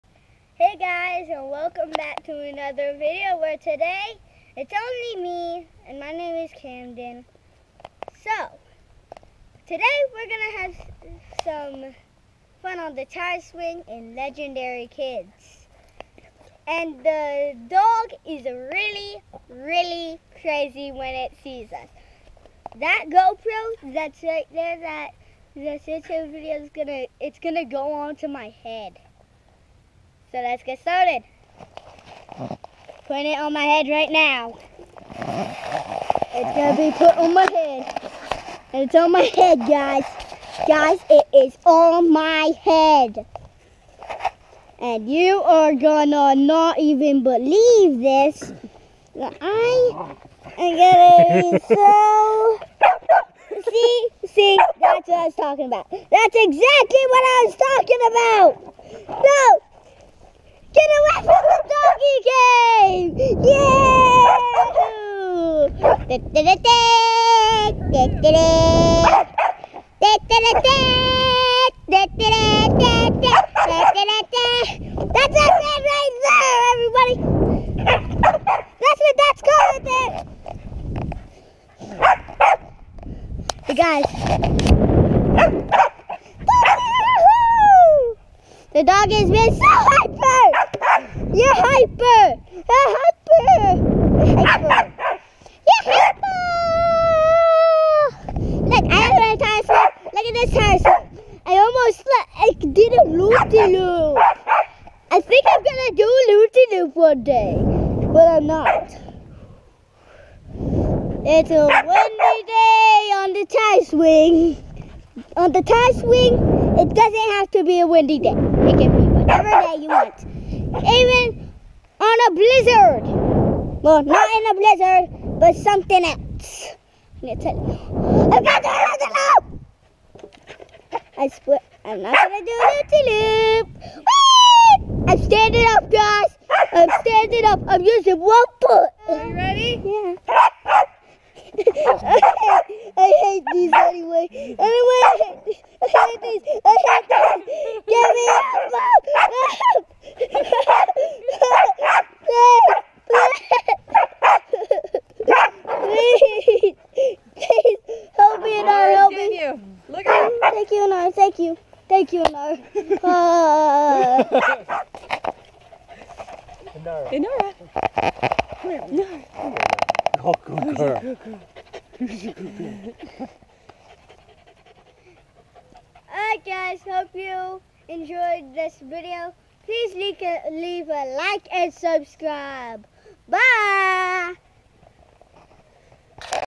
Legendary Kids Tire Swing Fun - Crazy Dog
Hey guys! Welcome back to another Legendary Kids Video! Just Camden here today! My Dog Inara Loves chasing me on the Tire Swing! Just some goofy fun and ramblings!